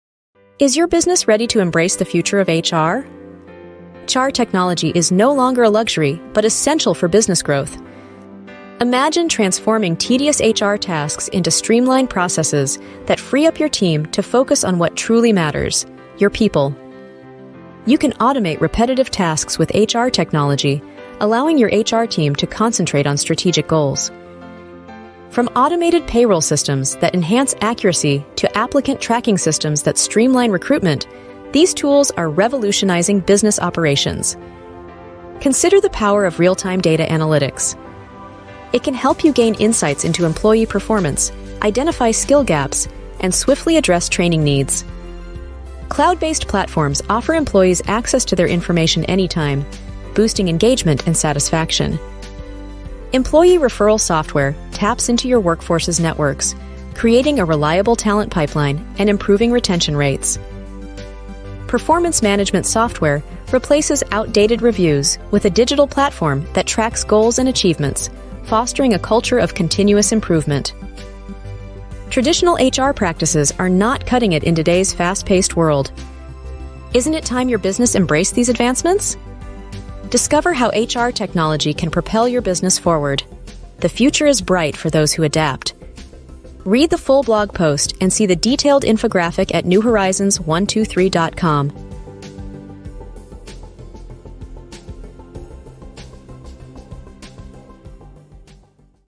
Why HR Technology is Essential for Business Growth
Is your business ready to embrace the future of HR?

Imagine transforming tedious HR tasks into streamlined processes that free up your team to focus on what truly matters: your people.

You can automate repetitive tasks with HR technology, allowing your HR team to concentrate on strategic goals.

From automated payroll systems that enhance accuracy to applicant tracking systems that streamline recruitment, these tools are revolutionizing business operations.

Consider the power of real-time data analytics.

It can help you gain insights into employee performance, identify skill gaps, and swiftly address training needs.

Cloud-based platforms offer employees access to their information anytime, boosting engagement and satisfaction.

Employee referral software taps into your workforce's networks, creating a reliable talent pipeline and improving retention rates.

Performance management software replaces outdated reviews with a digital platform that tracks goals and achievements, fostering a culture of continuous improvement.

Traditional HR practices are not cutting it in today's fast-paced world.

Isn't it time your business embraced these advancements?

Discover how HR technology can propel your business forward.

The future is bright for those who adapt.

Read the full blog post and see the detailed infographic